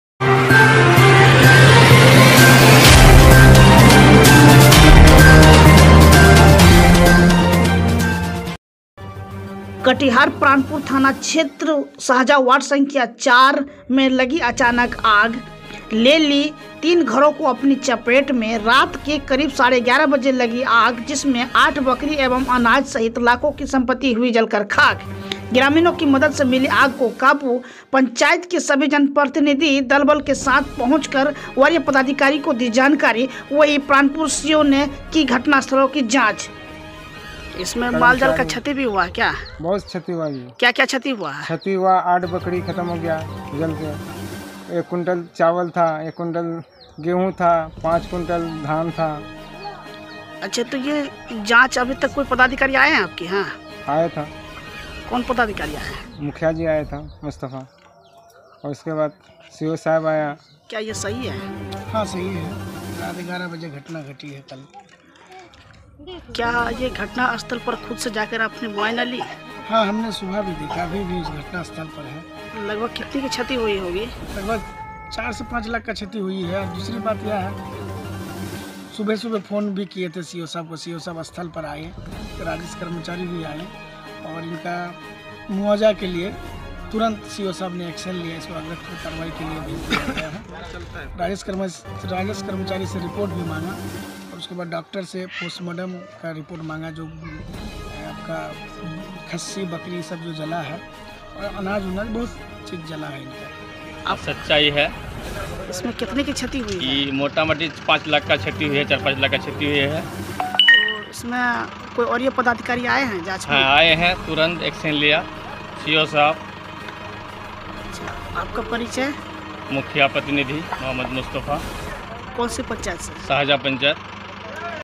कटिहार- प्राणपुर बेकाबू आग ने ली 8 मवेशी की जान लाखों की संपत्ति जलकर खाक
परिवारों में रो रो कर बुरा हाल || कटिहार प्राणपुर थाना क्षेत्र सहजा वार्ड संख्या 4 में लगी अचानक आग ले ली तीन घरों को अपनी चपेट में रात के करीब 11:30 बजे लगी आग जिसमें 8 बकरी एवं अनाज सहित लाखों की संपत्ति जलकर हुई खाक ग्रामीणों की मदद से मिली आग को काबू पंचायत के सभी जनप्रतिनिधि दल बल के साथ पहुंचकर वरीय पदाधिकारी को दी जानकारी वही प्राणपुर CO  घटनास्थलपर जा के की जांच